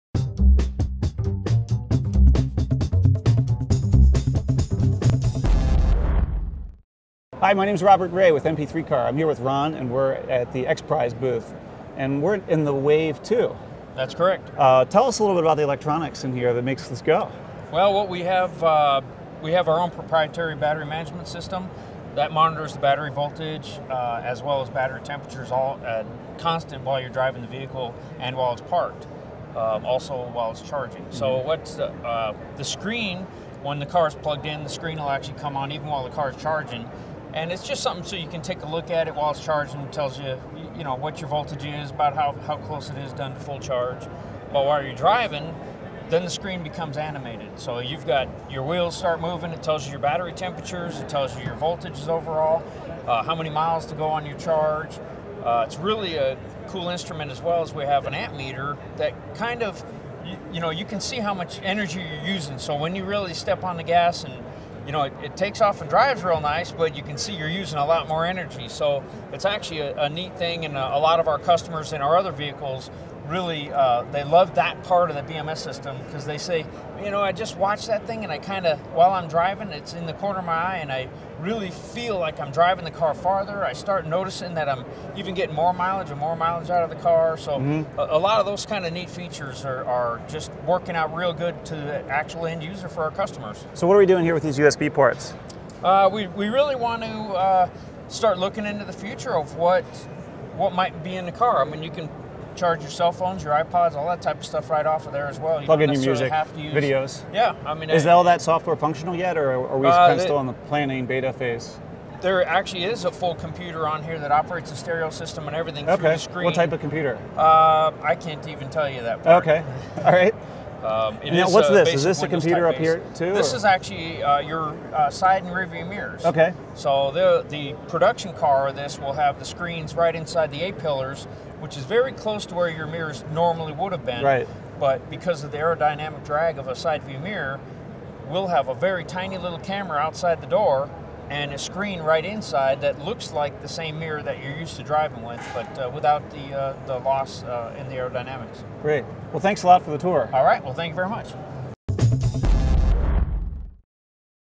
Wave 2 - an Electric X Prize Automobile - SEMA 2009
Rob Wray from mp3Car talks with Ron about the Wave 2. This car is a fully electric car with proprietary battery management system, complete with a ton of monitoring equipment.   The screen in the dash allows you to see the battery charging, battery temperatures, overall voltage, and how many miles to go on the charge.  The amp meter is alsodisplayed, so that you can see how much voltage you are using as you drive in order to learn how to drive more efficiently.

This car has a built in computer and USB ports to support the future of mobile electronics.

The Wave gets about 140 MPG!